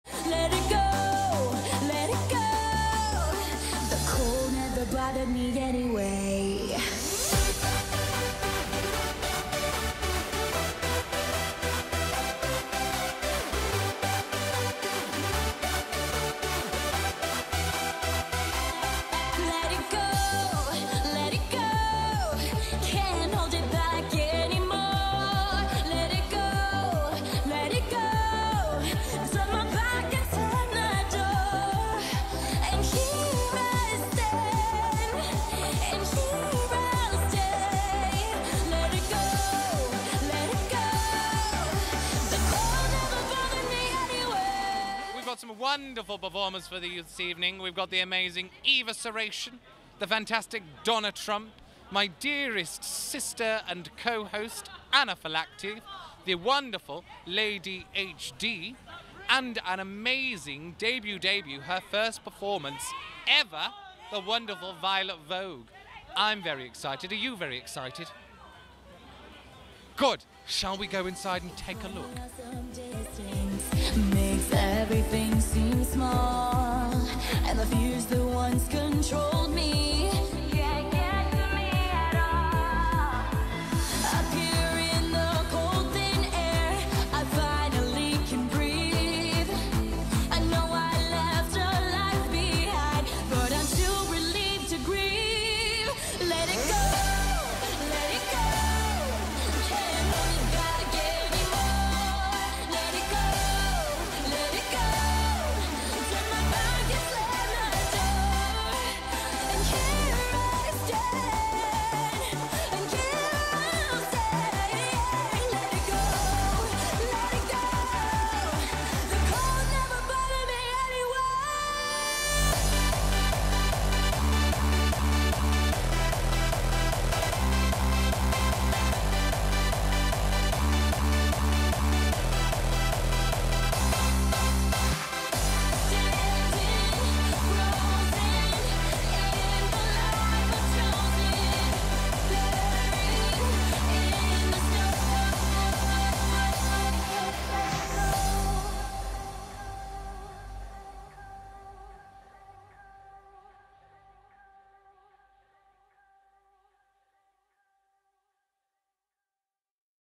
Disney Show Highlights | Cha Cha Boudoir - Canal Street Manchester
By popular demand this months Cha Cha Boudoir theme 'Wish upon a cha cha' see's you relive your beloved Disney / Pixar film characters. The best drag transformations we've seen yet! See who else you can spot - Mulan, Donald Duck, Mickey, Lumière, Cruella De Vil, Marlin and more!